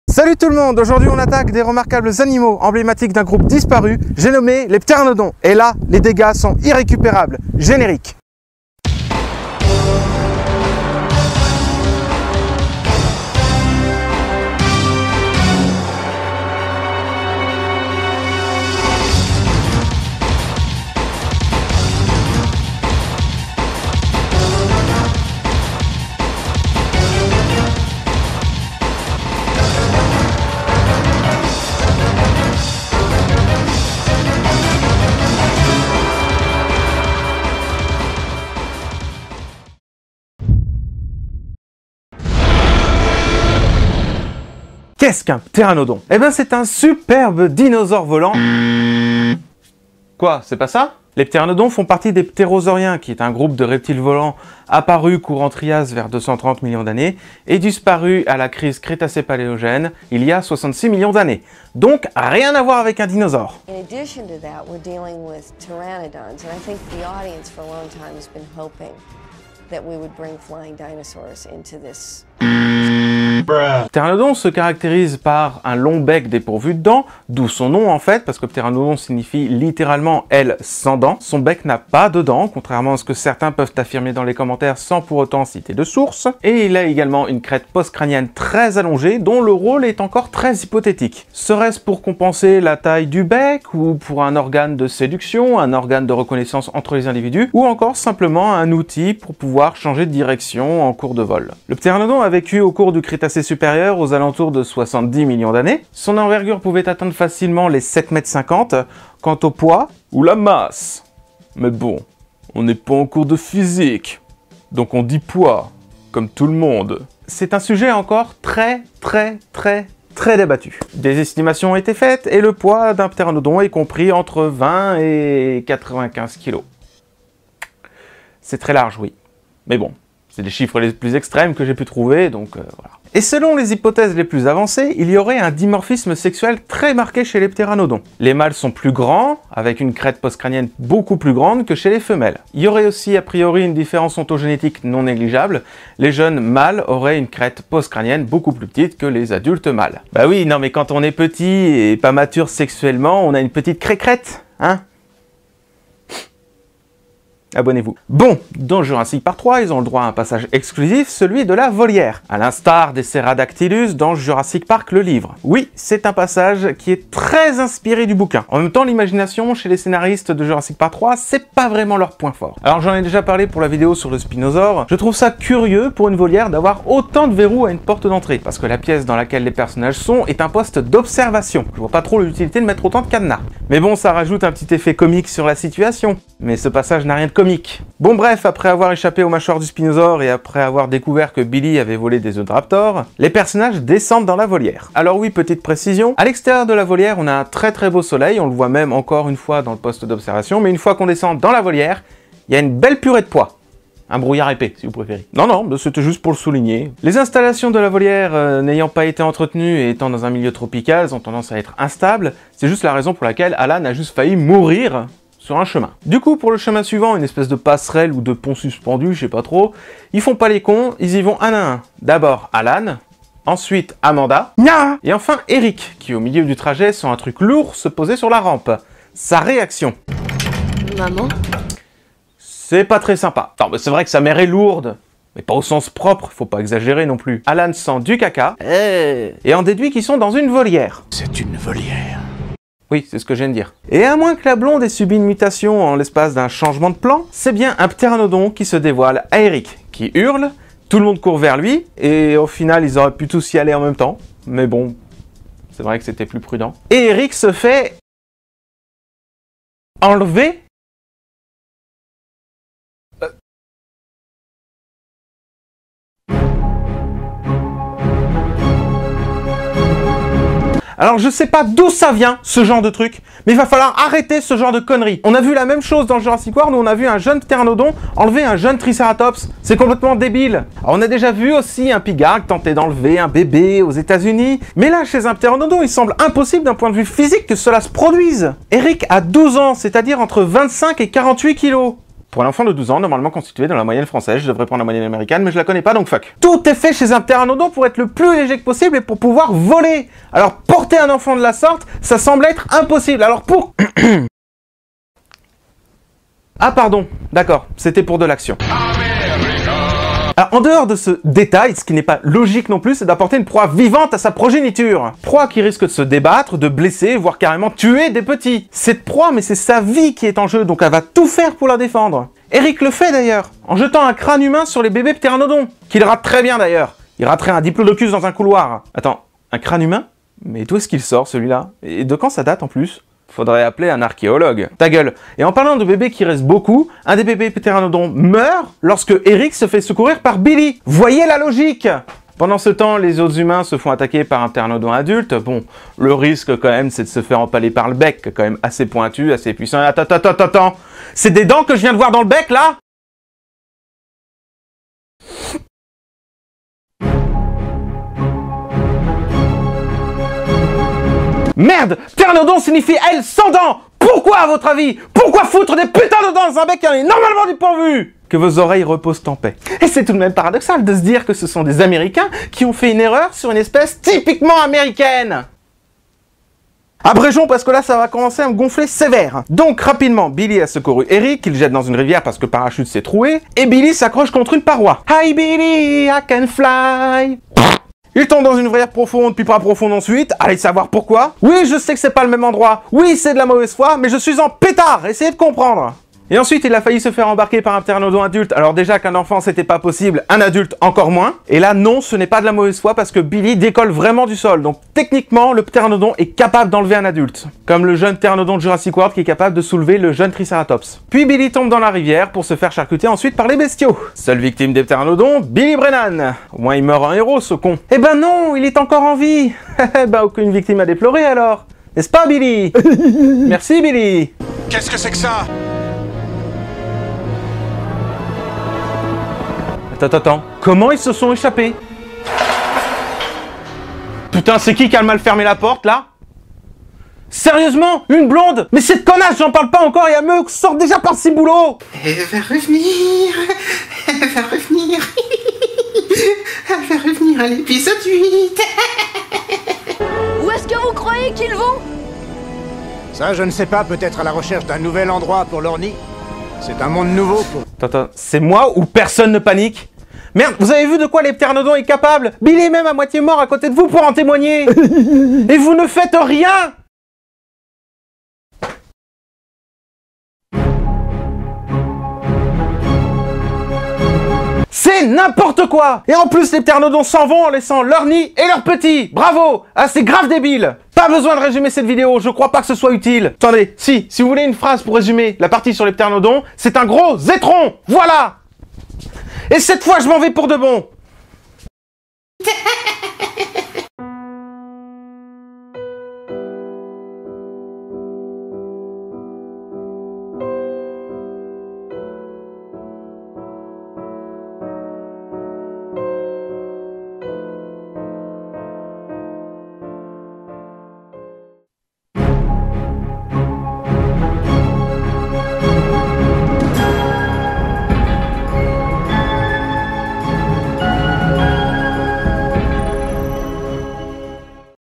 Critique de JP3 - Episode 04 : Les Ptéranodons
Salut la paléocommunauté !

Préparatifs : évitez les écouteurs pour cet épisode.

Aujourd’hui, 4ème épisode de la série de vidéos consacrée à la critique de Jurassic Park 3 (2001).

Nom d'un coprolithe ! Que ces animaux ont été massacrés !

J’espère que cet épisode vous plaira !

Que l’Évolution soit avec vous !